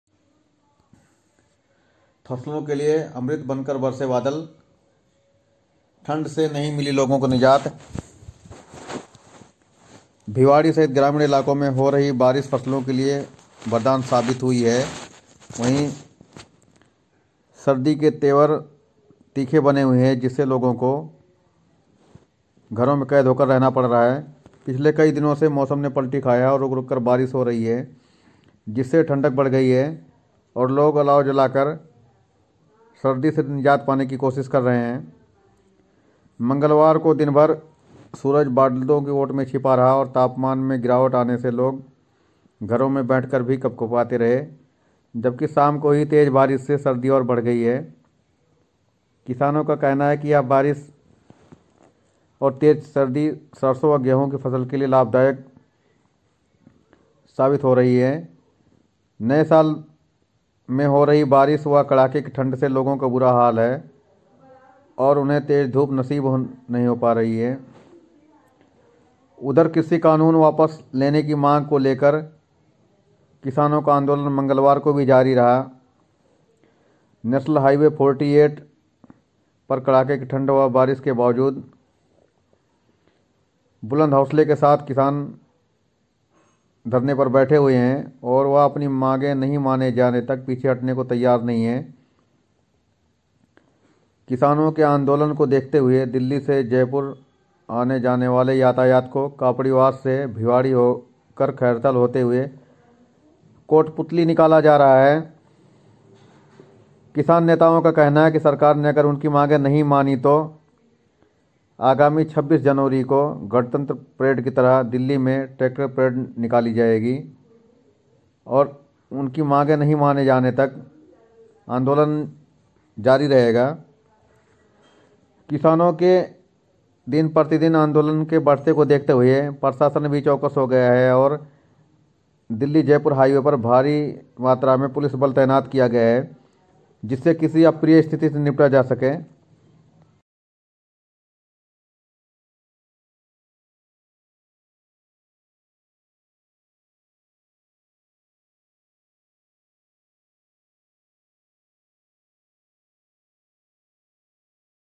फसलों के लिए अमृत बनकर बरसे बादल, ठंड से नहीं मिली निजात
भिवाड़ी। भिवाड़ी सहित ग्रामीण इलाकों में हो रही बारिश फसलों के लिए वरदान साबित हुई है। पिछले कई दिनों से मौसम ने पलटी खाया और रुक-रुककर बारिश हो रही है, जिससे ठंड बढ़ गई है और लोग अलाव जलाकर सर्दी से निजात पाने की कोशिश कर रहे हैं। मंगलवार को दिन भर सूरज बादलों की ओट में छिपा रहा और तापमान में गिरावट आने से लोग घरों में बैठकर भी कंपकंपाते रहे जबकि शाम को हुई तेज बारिश से सर्दी और बढ़ गई है। किसानों का कहना है कि यह बरसात और तेज सर्दी सरसों व गेहूं की फसल के लिए लाभदायक बताई जा रही है। नए साल पर हो रही बारिश व कड़ाके की ठंड से लोगों का बुरा हाल है और उन्हें तेज धूप नसीब नहीं हो पा रही है। उधर कृषि कानून वापस लेने की मांग को लेकर किसानों का आंदोलन मंगलवार को भी जारी रहा। कड़ाके की ठंड व बारिश के बावजूद किसानों के हौसले बुलंद हैं और वह अपनी मांगे नहीं माने जाने तक पीछे हटने को तैयार नहीं है। किसानों के आंदोलन को देखते हुए दिल्ली से जयपुर आने-जाने वाले यातायात को कापड़ीवास से भिवाड़ी होकर खैरथल होते हुए कोटपूतली निकाला जा रहा है।